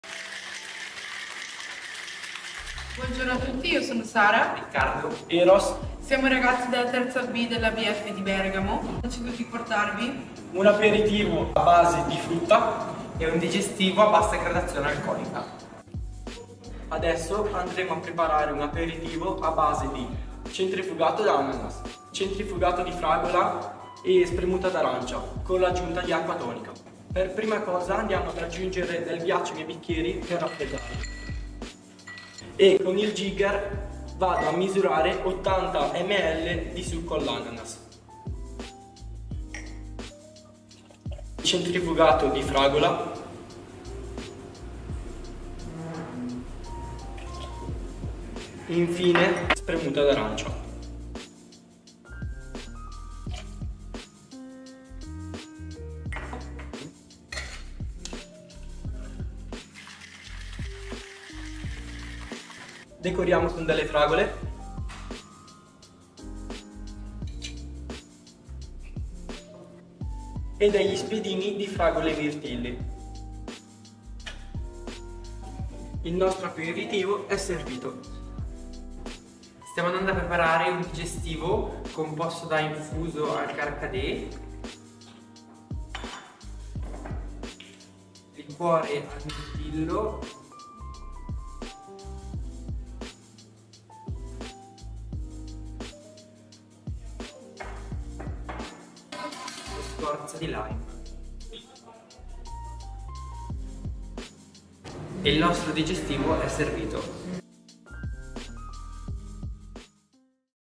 I Drink Primaverili di ABF
SOS ospiti in arrivo?
Non temere i ragazzi di Sala Bar di ABF ti aiuteranno nella realizzazione di semplici cocktail perfetti per accogliere i tuoi amici.

0:16 Analcolico fruttato
1:18 Digestivo all'infuso di karkadè